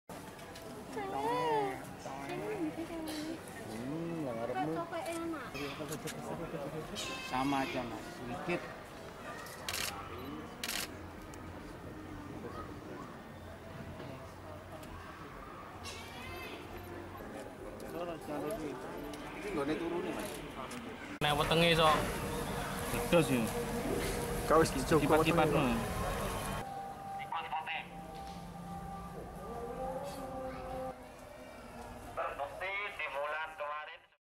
Dozens Of Komodo Dragon Babies Hatch At Indonesian Zoo
March 06, 2019 - Over 70 Komodo dragon babies join Surabaya Zoo in Indonesia's East Java province, hatched from seven different female Komodo dragons.